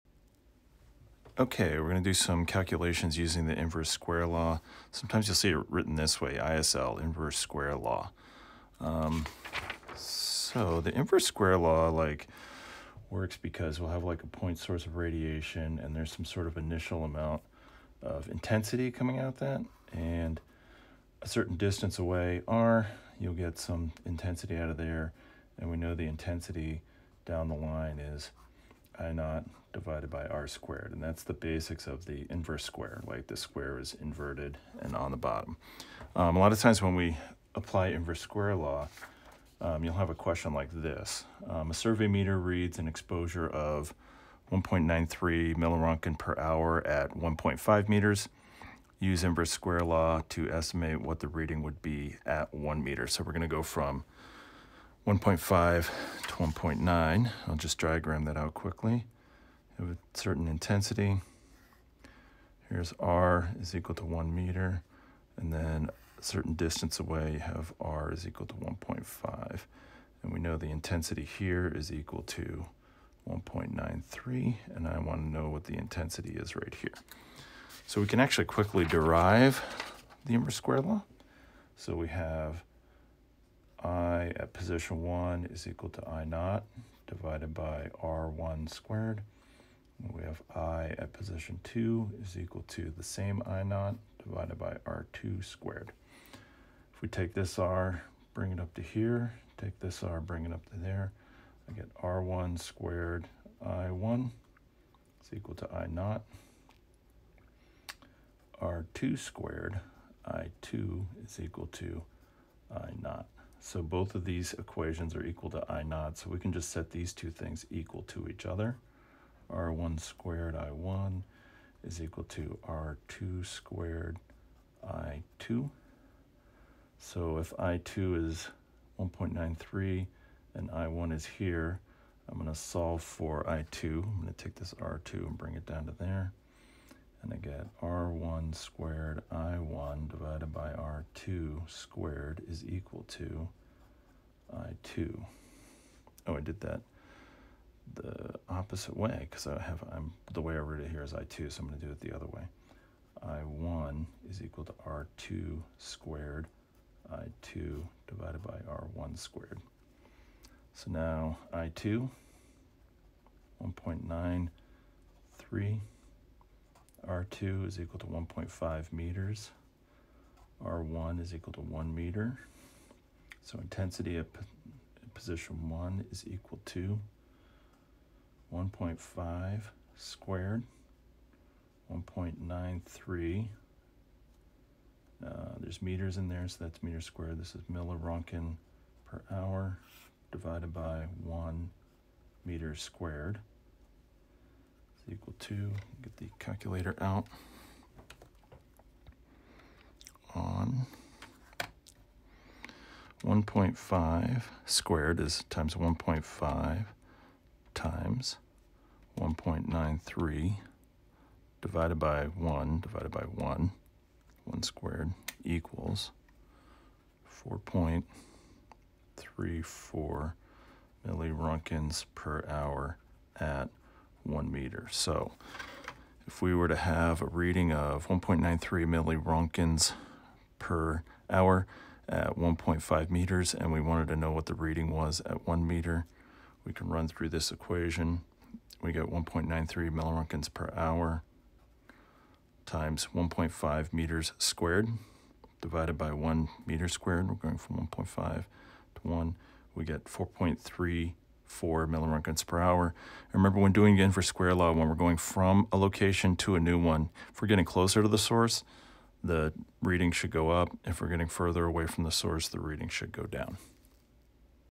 09 Calculation - Inverse Square Law ISL
Sample calculation using inverse square law.